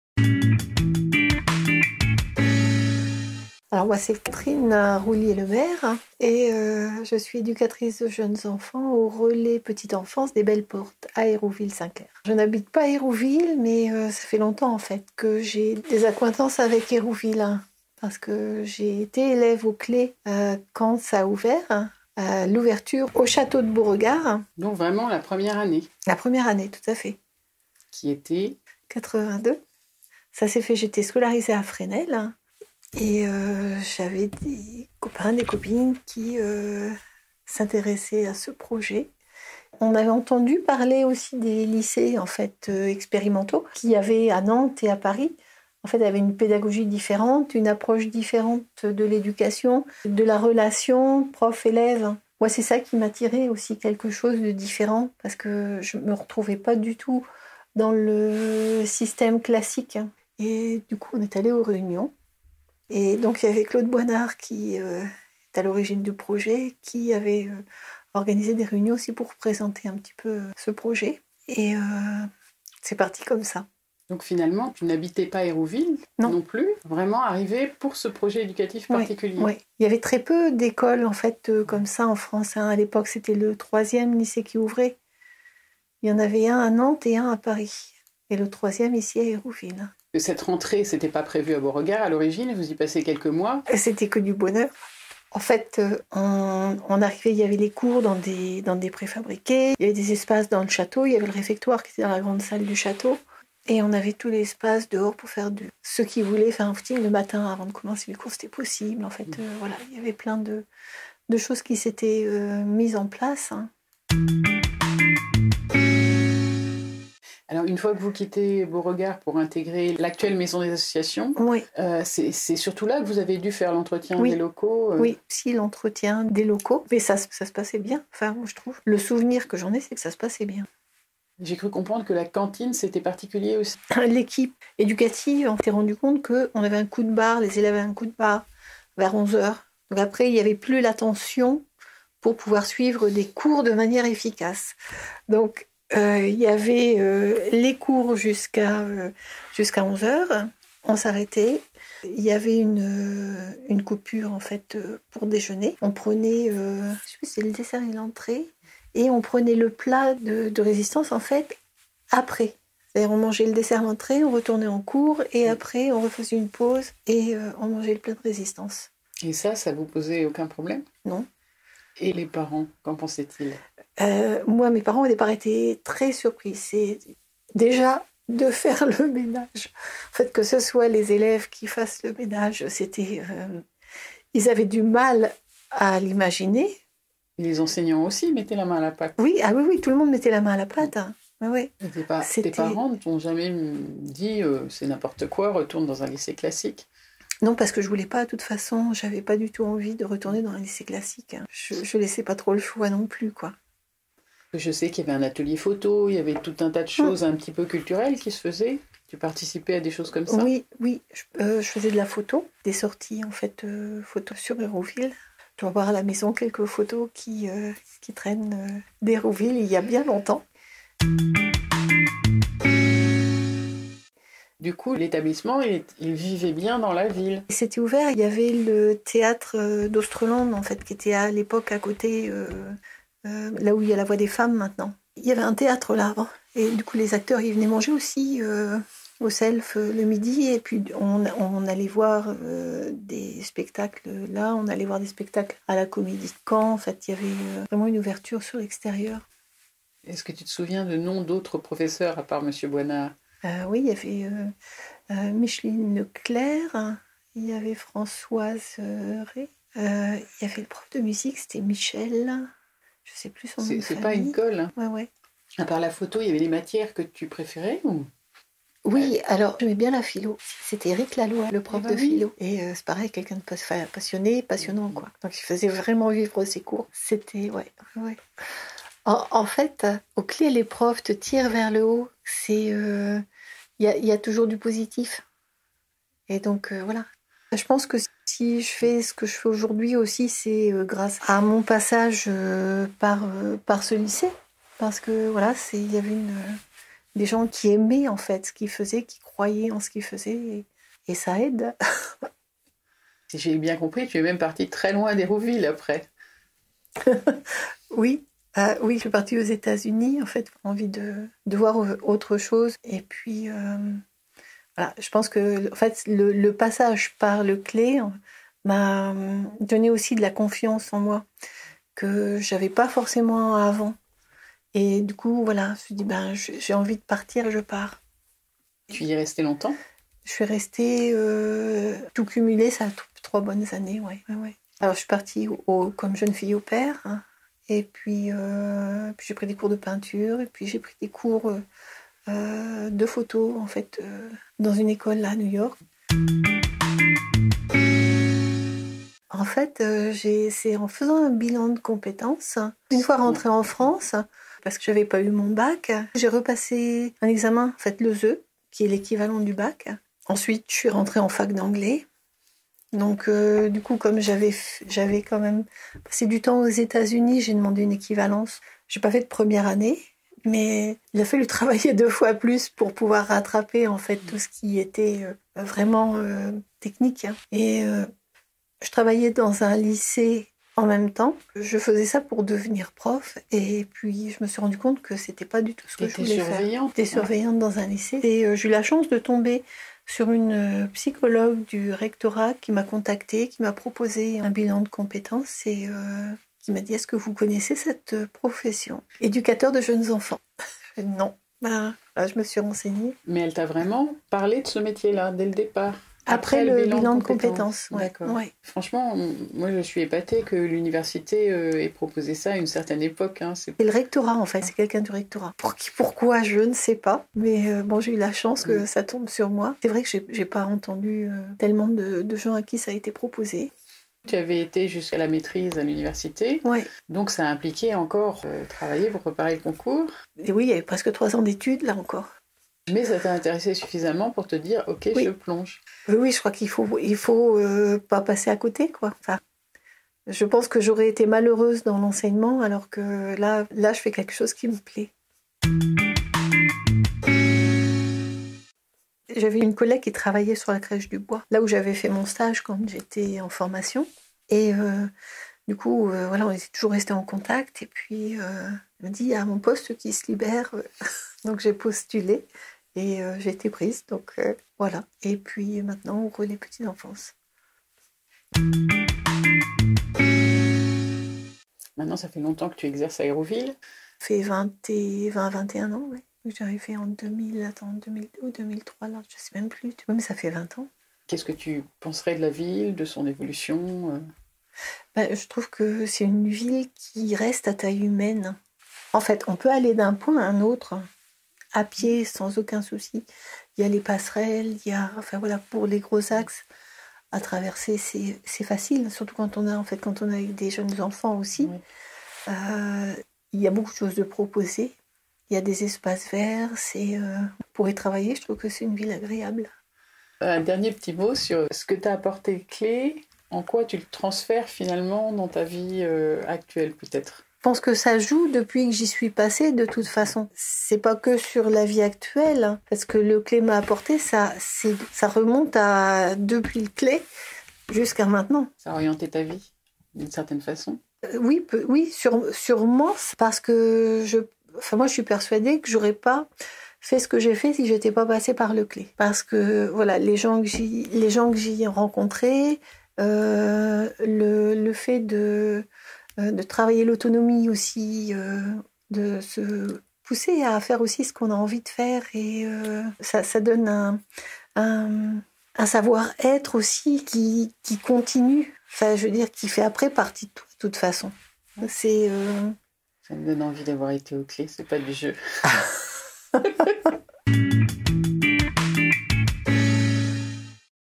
1 PODCAST - Catherine ROUILLIER-LEMARIE- Expo "Mémoire vivante d'Hérouville Saint-Clair"
Elève de la première promotion du Collège Lycée Expérimental (CLE).

Témoignages recueillis en 2022 et 2023 - Service des archives de la Ville - dans le cadre du 60e anniversaire de la Ville Nouvelle

Exposition "Mémoire vivante d'Hérouville Saint-Clair"
Initiée par le service des archives de la Ville, cette exposition présente des témoignages d’habitants et d’acteurs du territoire sur leur vécu à Hérouville Saint-Clair et l’évolution de la ville, recueillis lors d’entretiens. Elle donne lieu à des podcasts et à une exposition sous forme de 30 panneaux 60x80 cm. Cette exposition itinérante sera notamment visible au Pôle séniors et dans le hall de l’Hôtel de Ville au mois de novembre 2023.